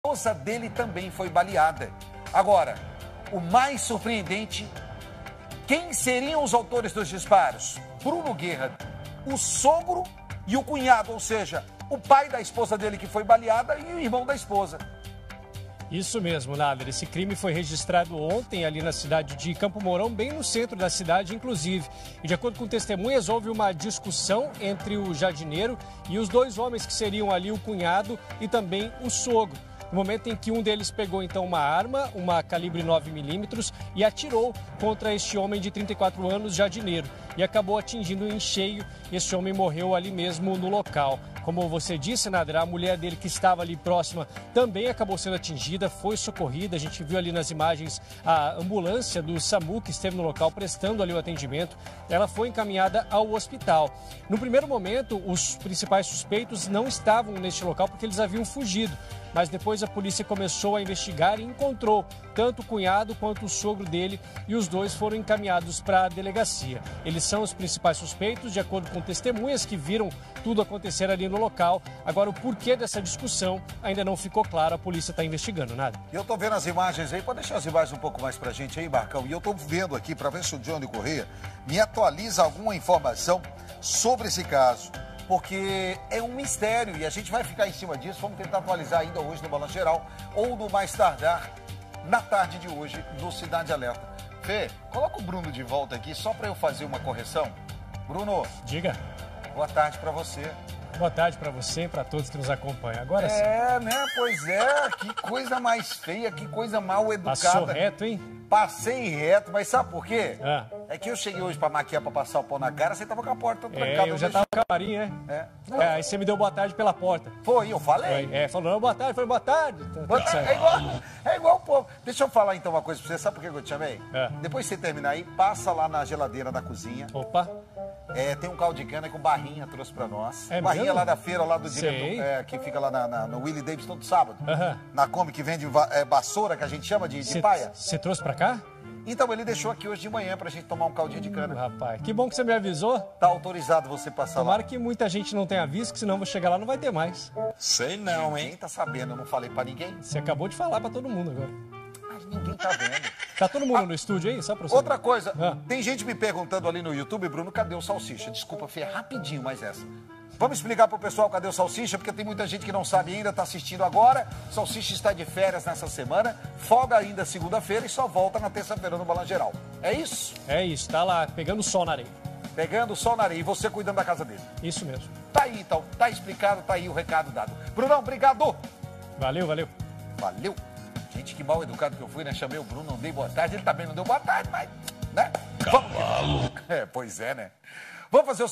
Após discussão homem é morto a tiros pelo sogro w cunhado em Campo Mourão
Mais um homicídio foi registrado em Campo Mourão no início da tarde de hoje, o segundo em menos de 48 horas e o 20º do ano. A vítima é o jardineiro Bruno Fermino dos Santos, 34 anos. A esposa dele também foi alvejada e foi socorrida pelo Samu. Ele foi morto com tiros de pistola 9mm, e o autor do crime fugiu do local. O homicídio ocorreu no centro de Campo Mourão.